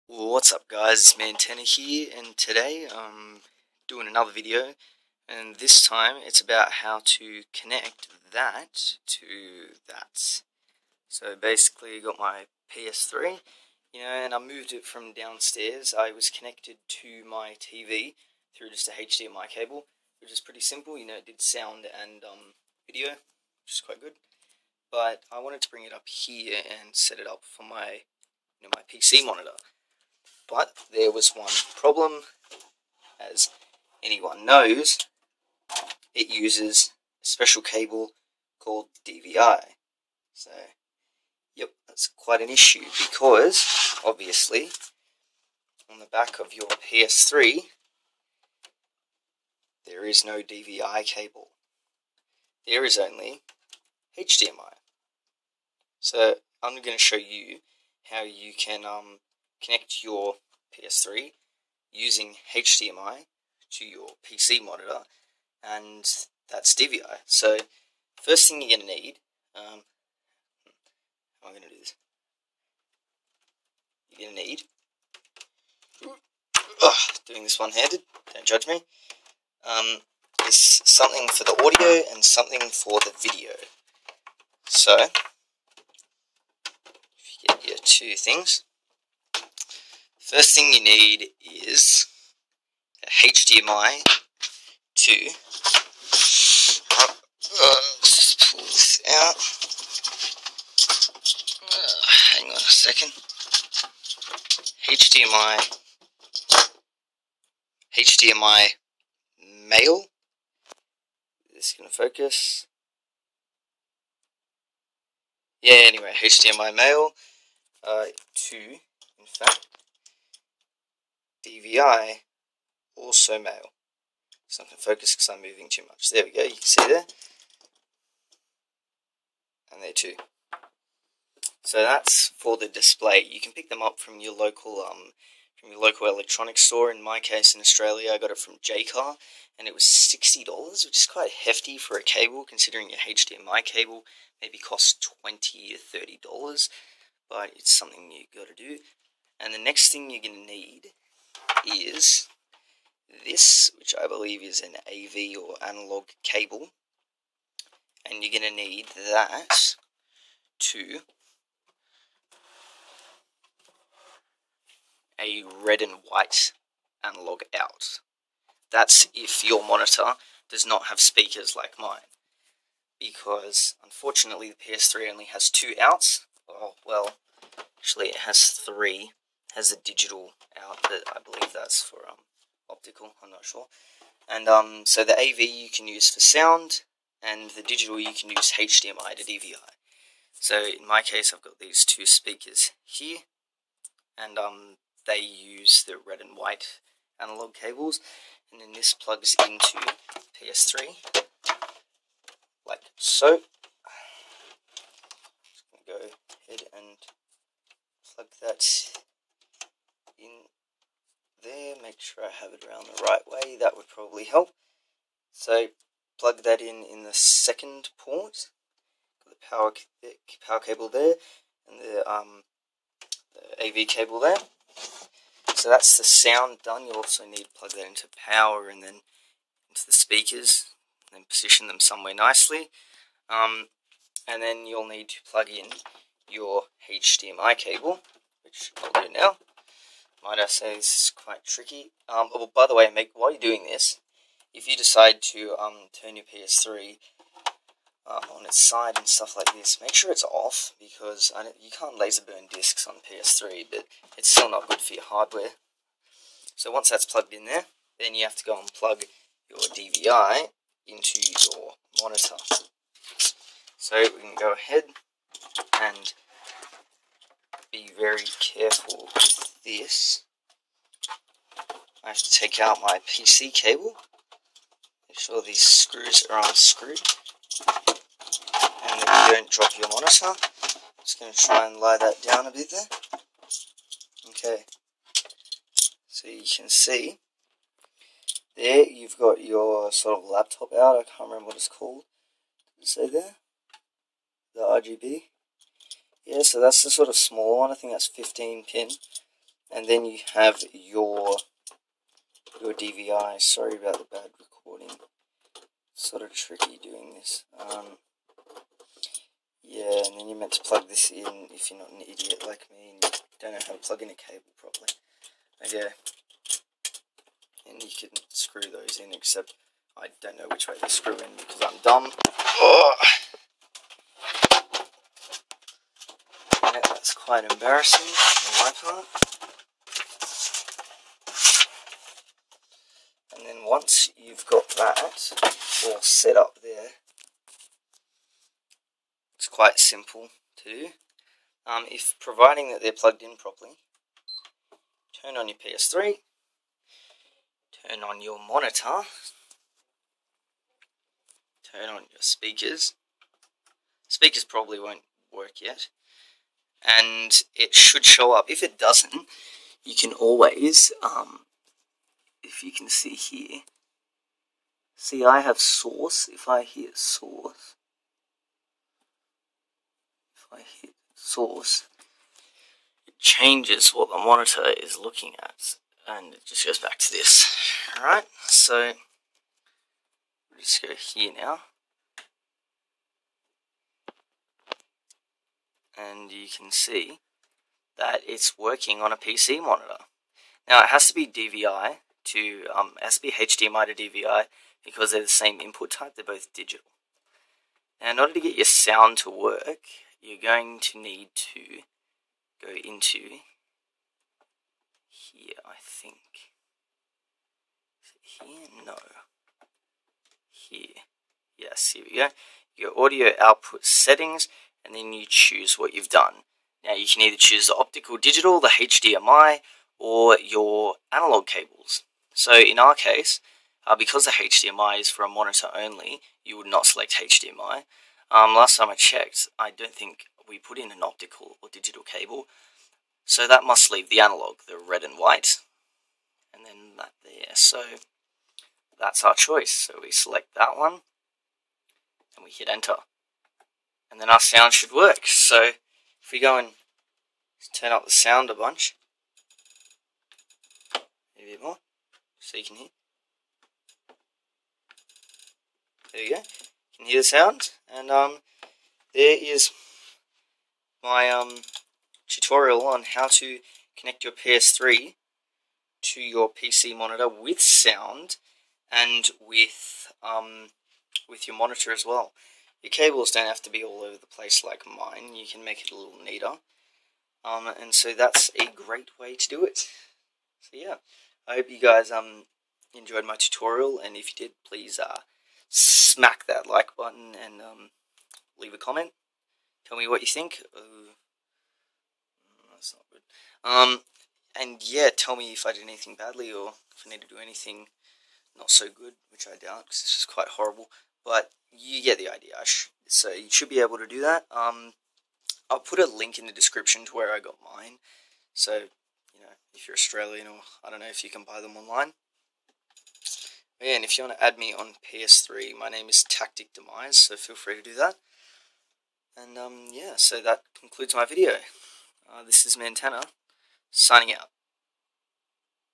~ How to ~ Connect your ps3 (or Xbox) to a PC monitor
In this video I show you how to connect your ps3 (or xbox in the same way) to your pc monitor using a HDMI male to DVI male cable that you can pick up from your local electronic store. For your convenience I have provided a few links, including the one where I bought the cable from below. THANKS